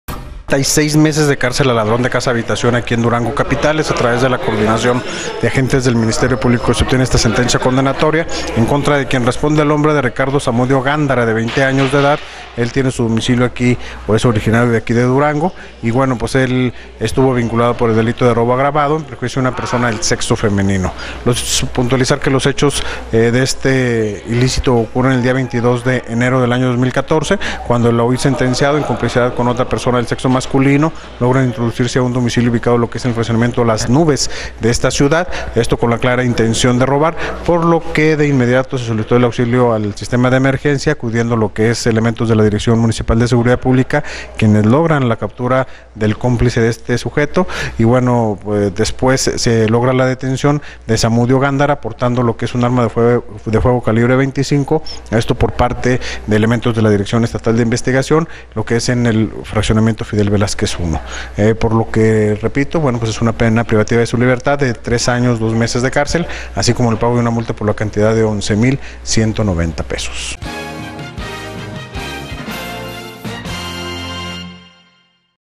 Más de tres años a la cárcel por robar
Ricardo Zamudio Gándara de 20 años de edad, con domicilio en la ciudad de Durango, por el delito de robo agravado en perjuicio de una persona del sexo femenino, quien el pasado día 22 de enero del año 2014, cometió el ilícito junto a su cómplice se introdujeron a una casa  ubicada en el fraccionamiento Las Nubes de la capital.